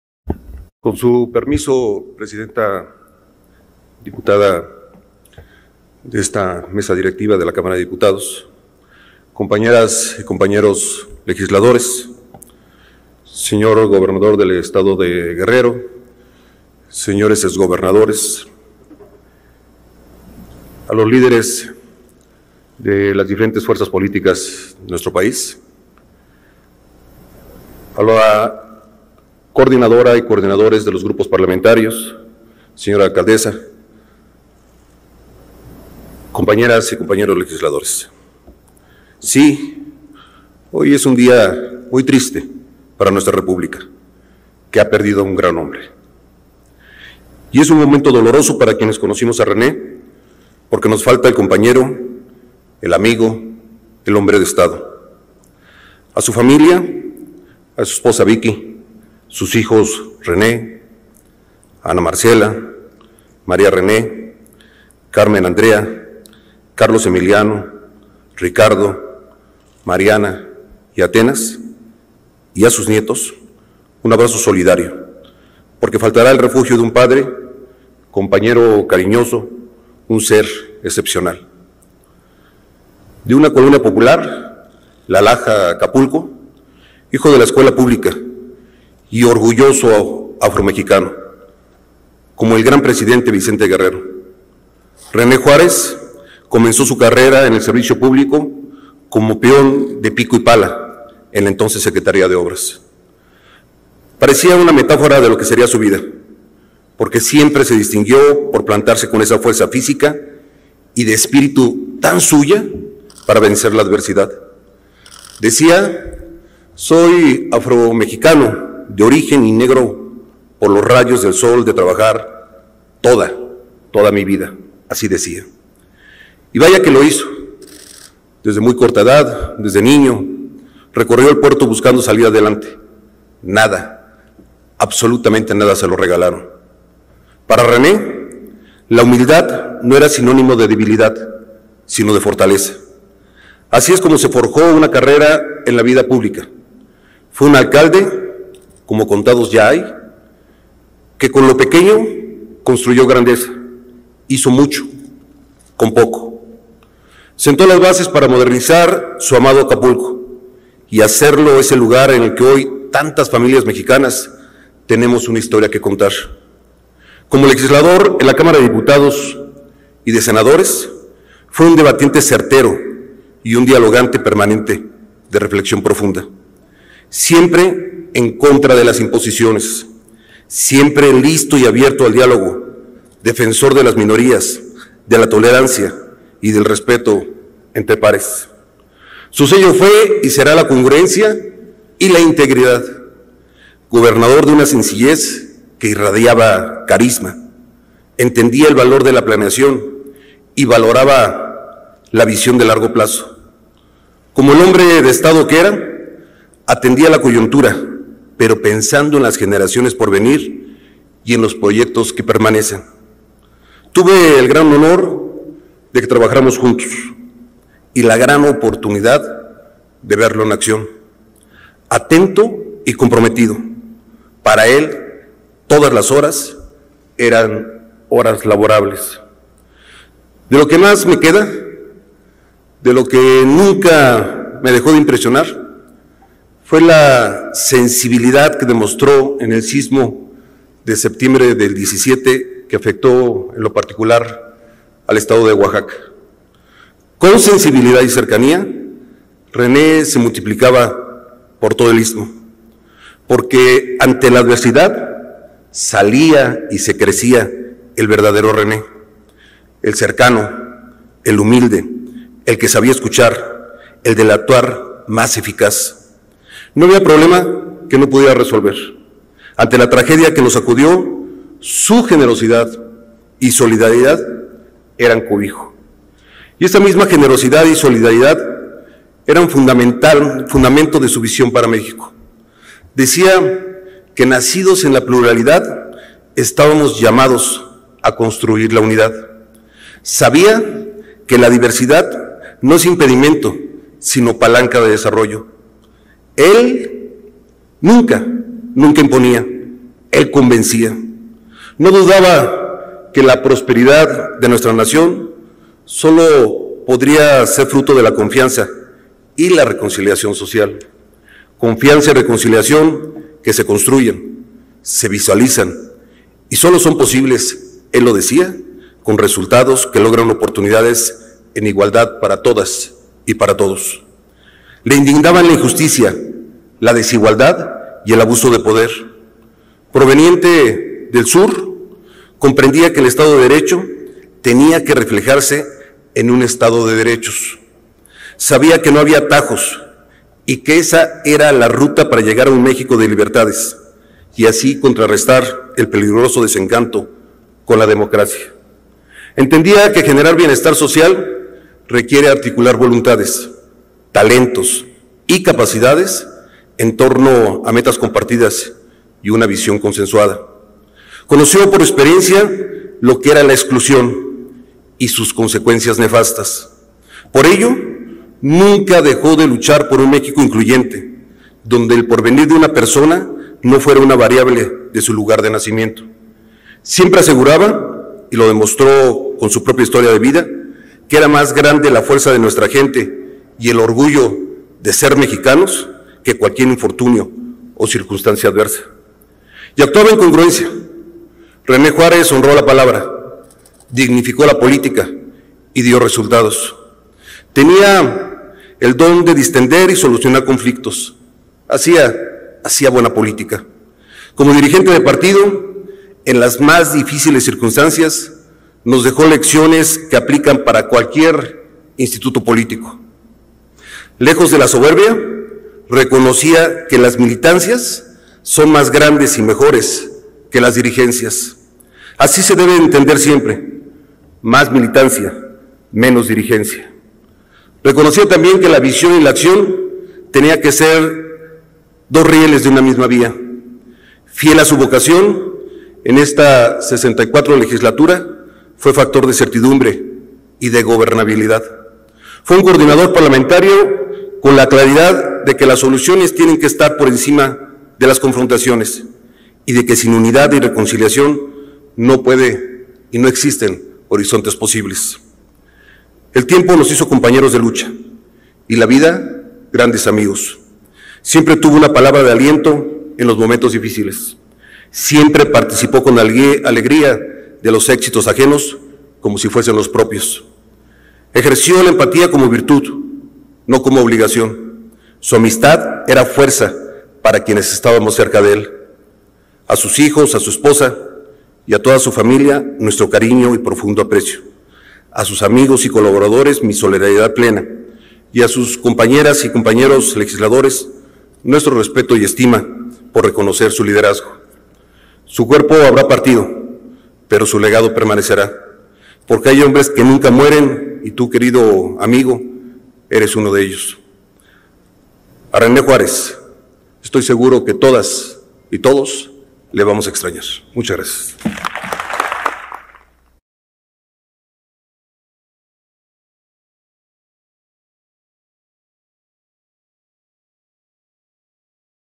Sen. Miguel Ángel Osorio Chong (PRI) / Homenaje luctuoso del diputado René Juárez.
El Sen. Miguel Ángel Osorio Chong, coordinador del Grupo Parlamentario del PRI en el Senado de la República, señala que para René Juárez Cisneros la humildad no era sinónimo de debilidad, sino de fortaleza. Fue un debatiente certero y un dialogante permanente, defensor de minorías y respeto entre pares.